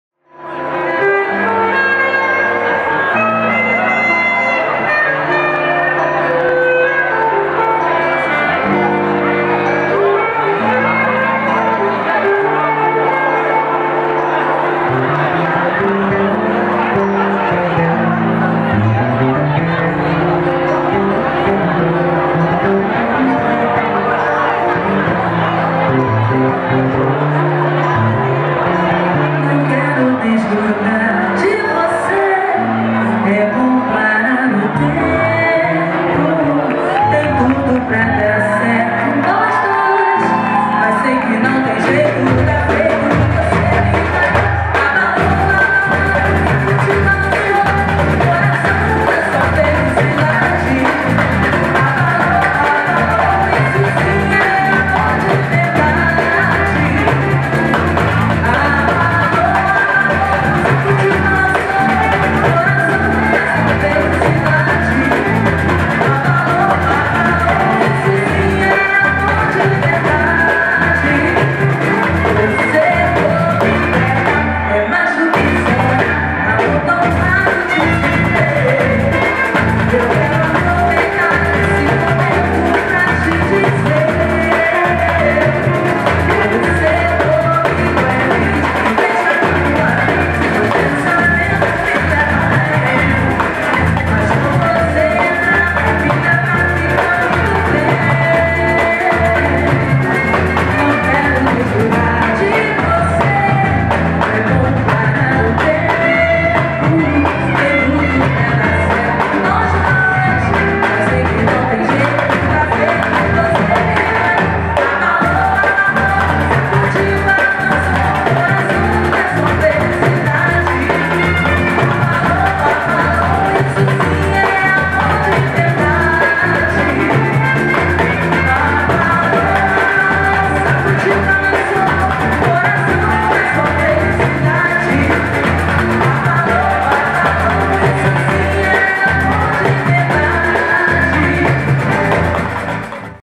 Um Amô na Feijoada do Salgueiro
Um Amô se apresenta na feijoada do Salgueiro do dia 11 de junho de 2017.

Imagens e edição: Rafael Arantes.